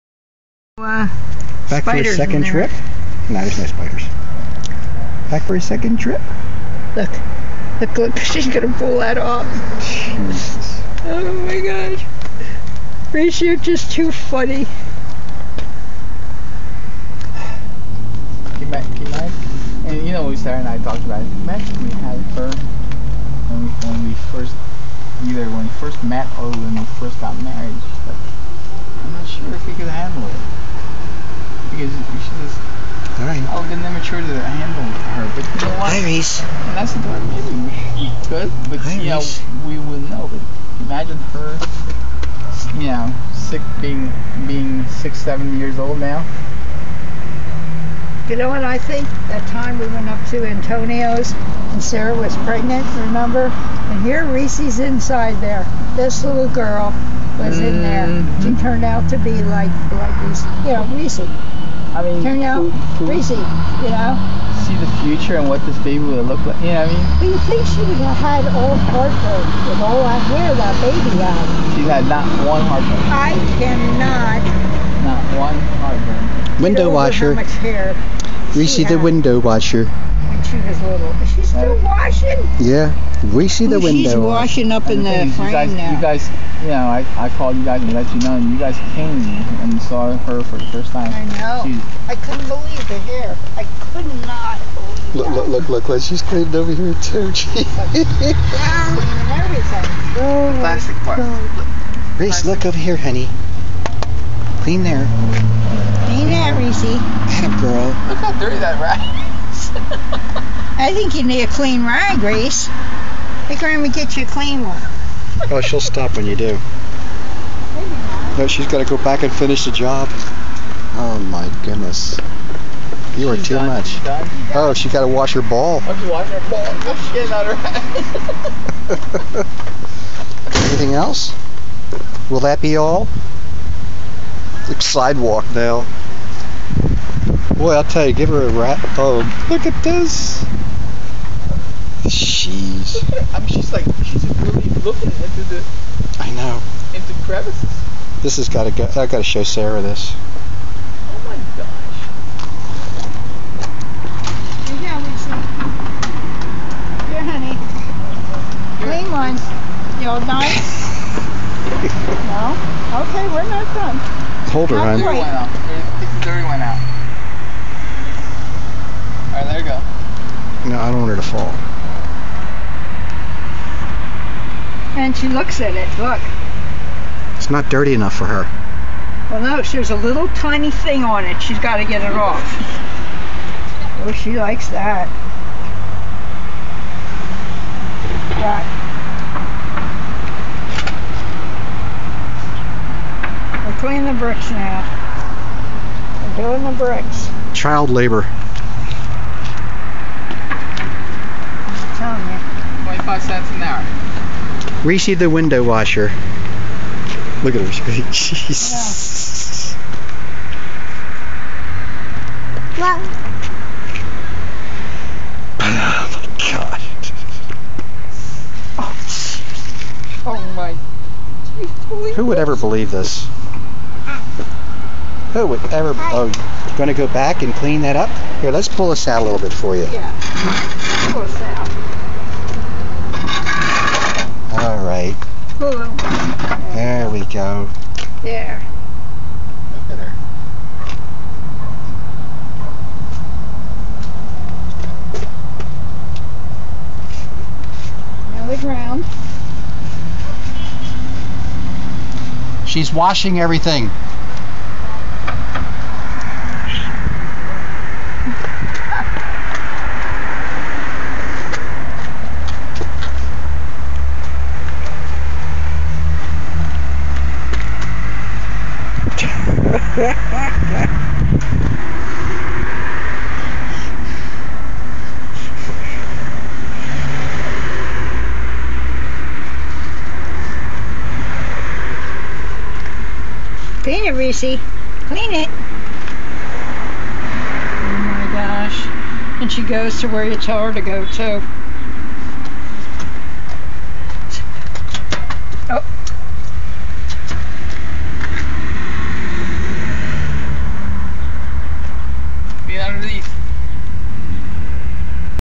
Reesies window & general cleaning. Low rates. Quality work.
Who would believe that this 18 month old little sweetheart has this much interest in keeping things clean. I do. So does her daddy, and grammy. Now we have proof for all to see. God love her, she is so unbelievably grown up for her age. She also made a new friend today.